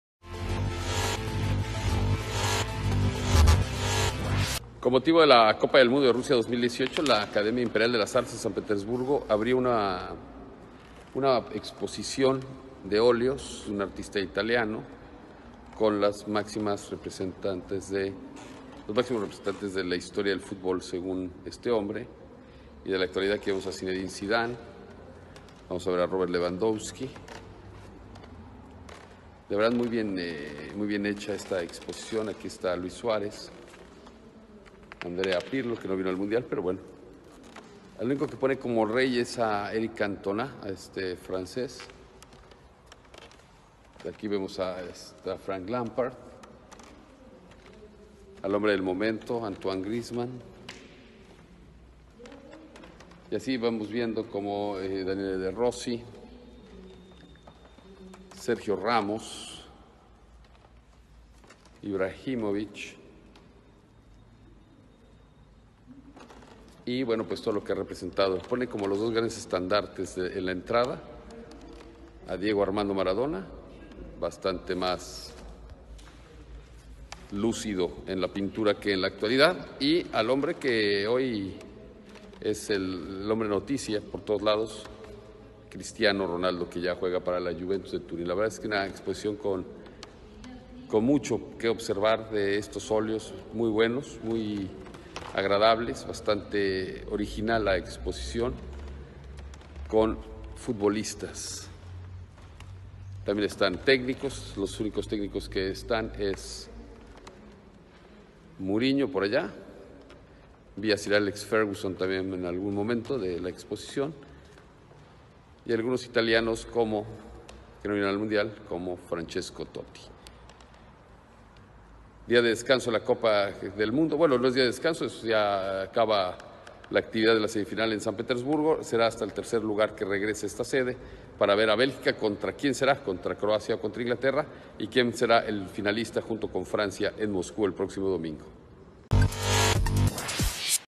Exposición de óleos de artista italiano en Rusia
Grandes figuras del futbol aparecen en la exposición que se presenta en la Copa del Mundo